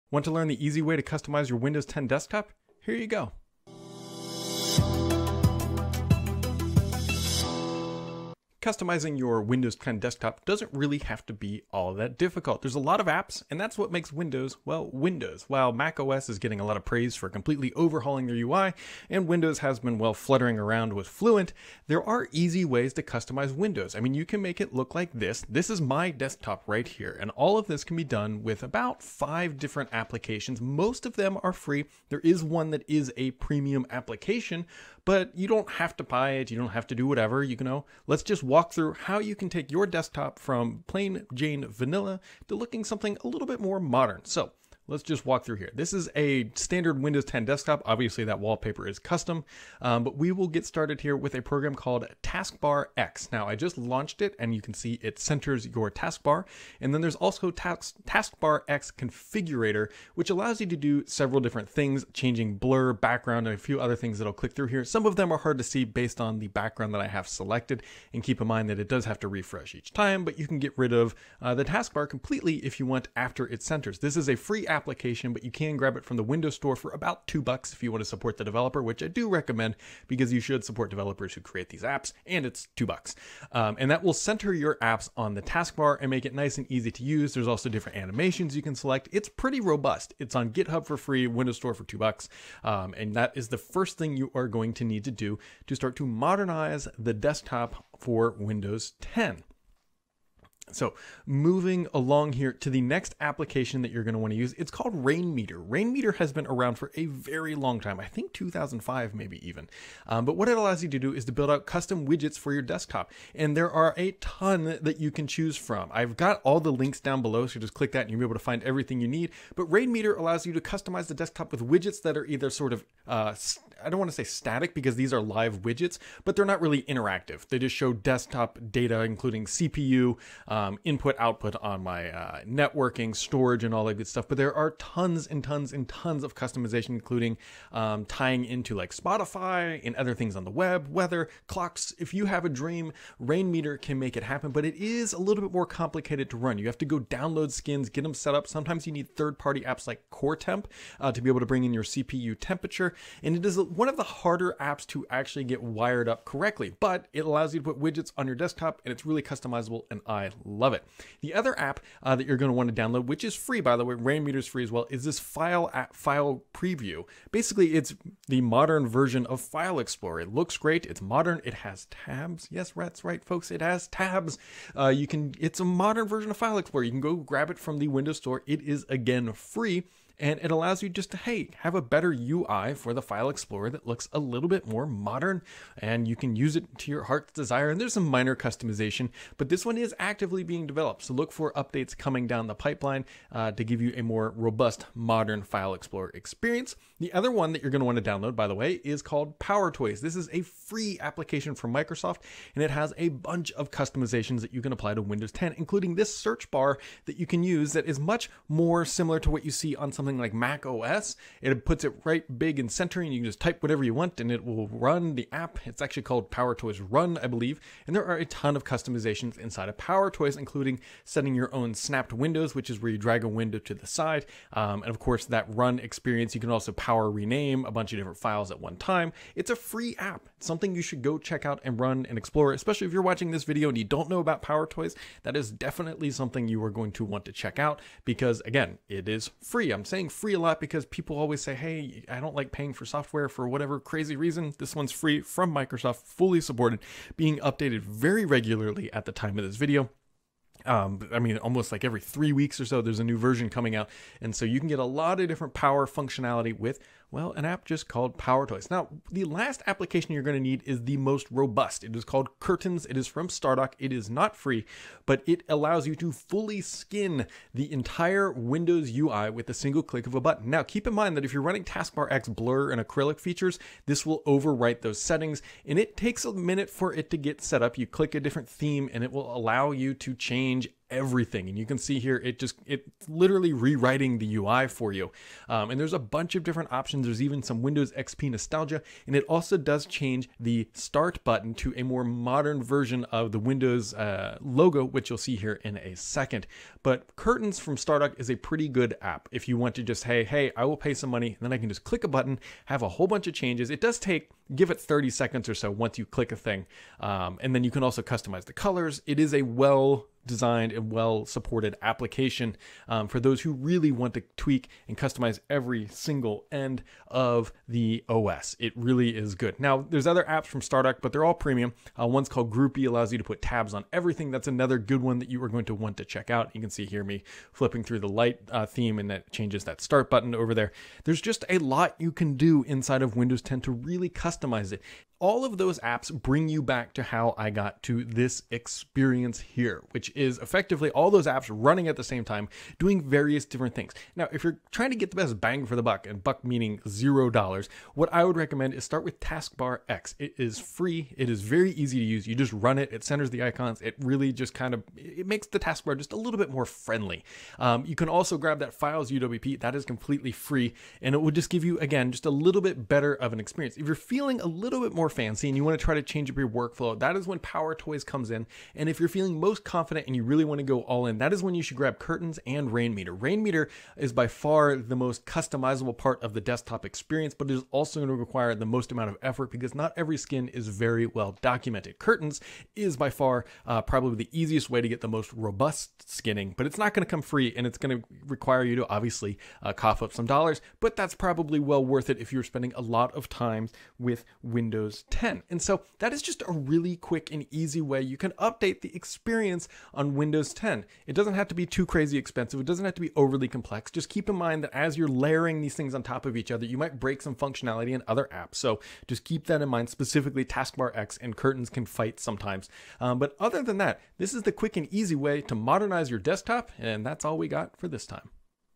Easy Way To Customize Windows 10 for Free
Looking to modernize your Windows 10 desktop, here is the easy way to do it for free.